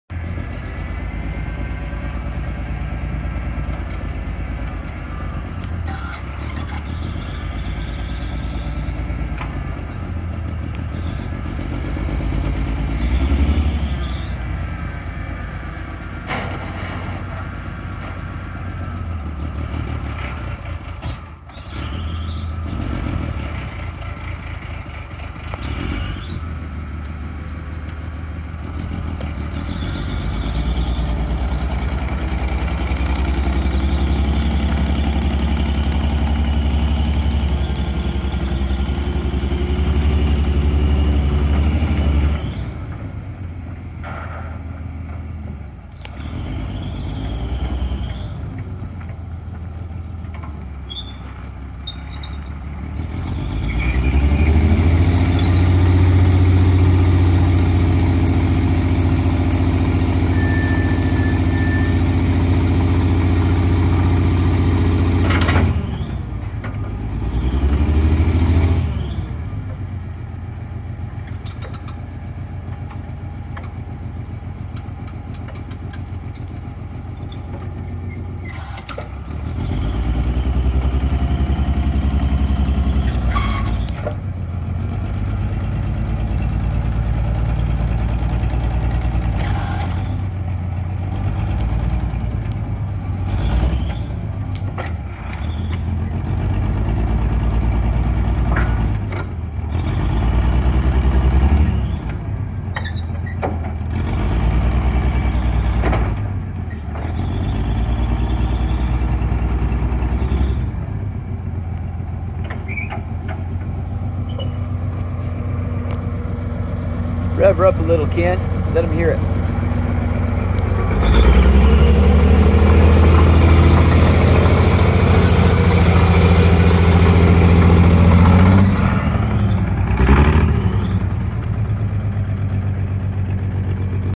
Hyster B70 forklift, V6 engine, w/ Sunny D bale squeeze
Jack and Bonnie Smith Estate Farm Equipment Auction